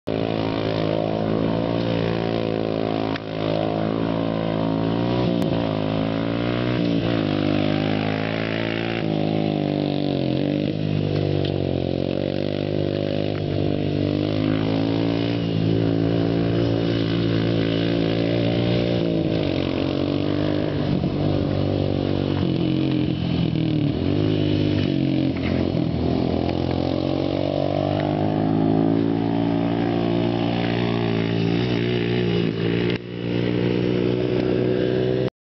hmf pipe honda foreman 450
honda foreman with a hmf pipe wide open